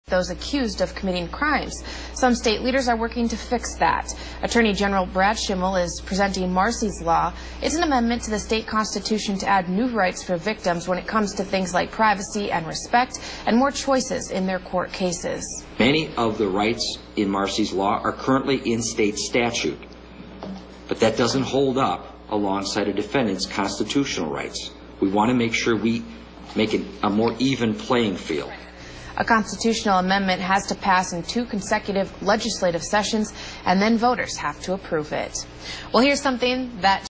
Attorney General Brad Schimel Supports Marsy's Law For Wisconsin - WQOW
Wisconsin Attorney General Brad Schimel appeared alongside others to give support for Marsy's Law for Wisconsin, which would give victims of crime in the state equal rights in the criminal justice system.